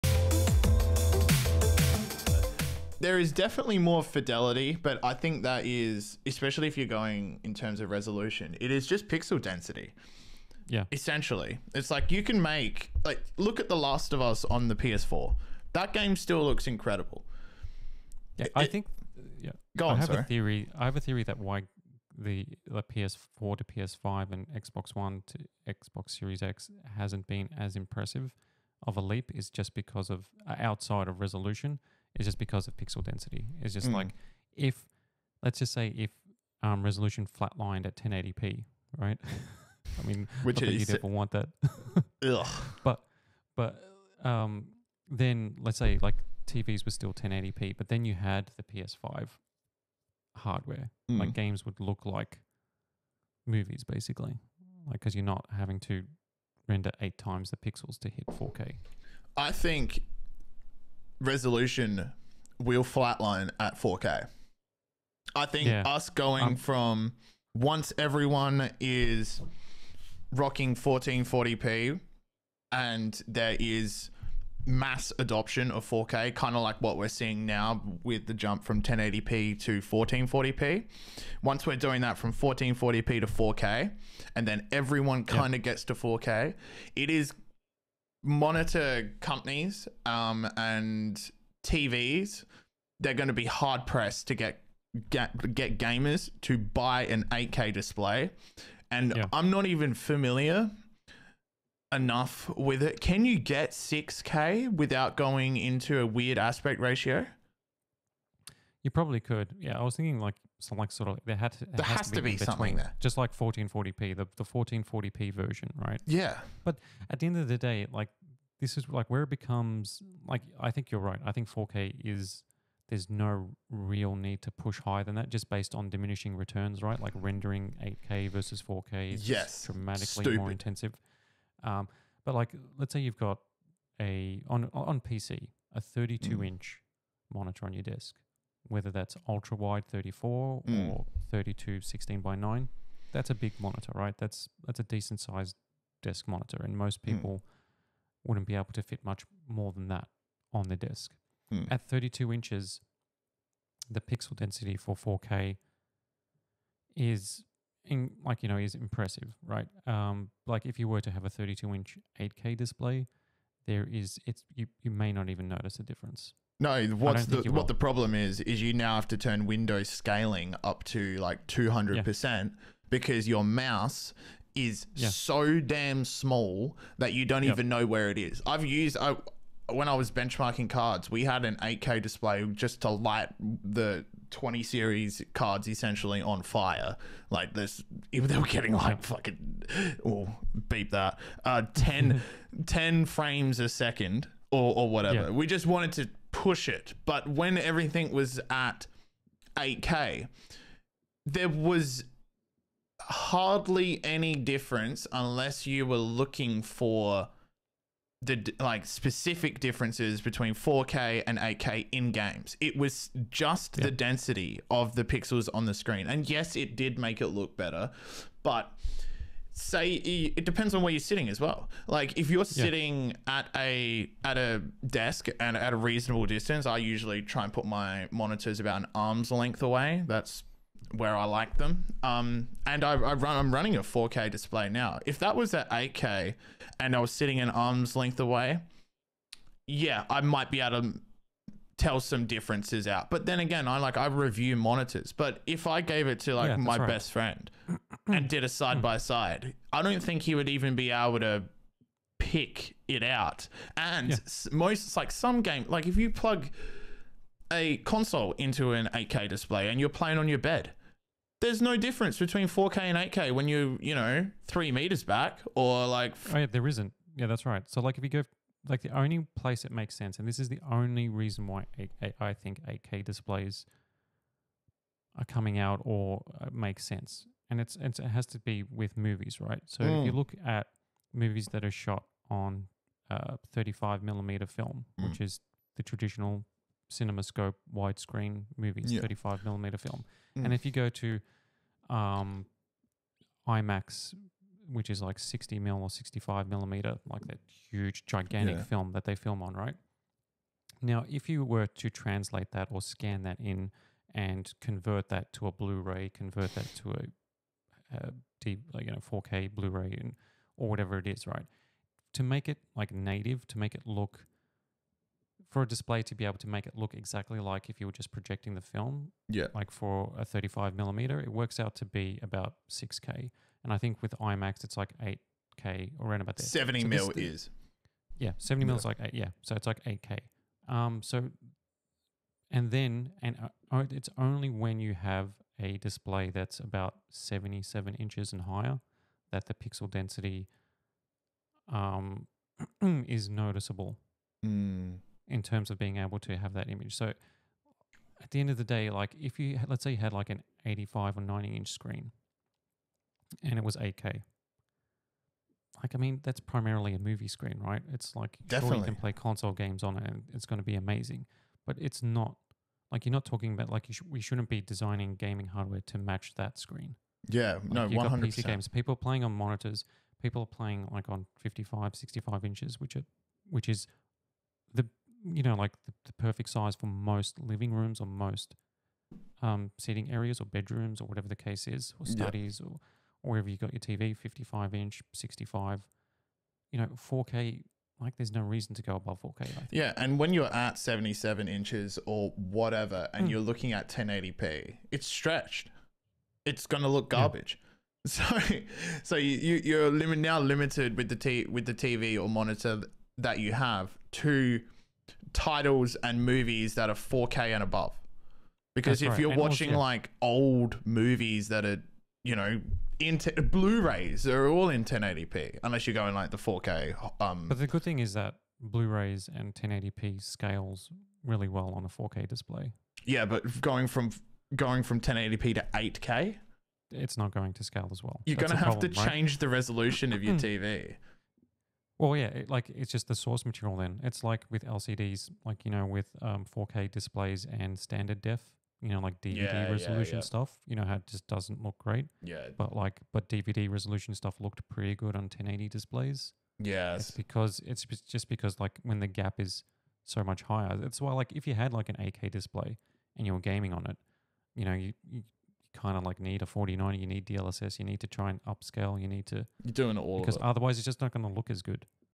Why We Don't Need 8K Gaming
With native 4K rendering at super-high frame-rates still out of reach for most gaming hardware, is the push for 8K a little too premature? Even for the upcoming PlayStation 6?